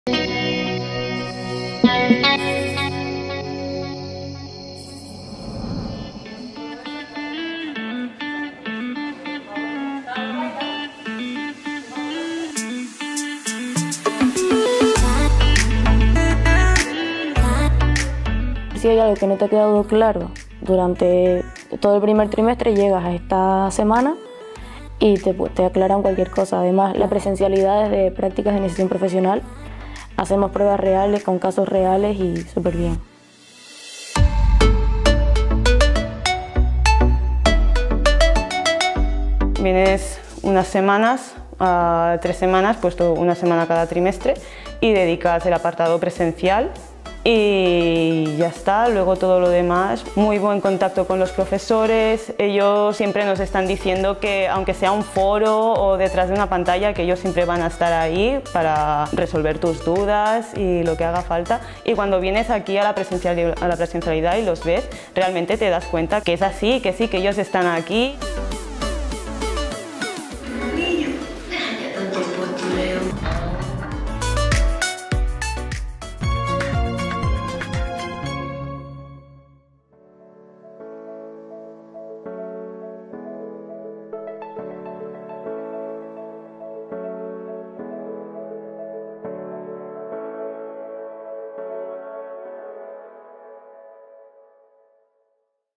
Prácticas presenciales del Grado en Ciencias de la Actividad Física y del Deporte (10/2018)
¿Te apasiona el deporte? ¡Infórmate sobre el Grado en Ciencias de la Actividad Física y del Deporte (CAFD) de la Universidad Isabel I!

Vídeo resumen de las prácticas presenciales del Grado en Ciencias de la Actividad Física y del Deporte (CAFD) en el Centro de Investigación en Ciencias de la Actividad Física y del Deporte del Edificio de I+D+i de la Universidad Isabel I, en las que los alumnos de la titulación desarrollan los conocimientos teóricos adquiridos en los laboratorios equipados con tecnología de vanguardia de la institución académica, que les ofrece la posibilidad de adaptarse a sus necesidades y horarios en función de las obligaciones de cada estudiante. Por otra parte, también han realizado algunas dinámicas en la Academia Hélade dado que se adaptaban mejor a las necesidades de las prácticas.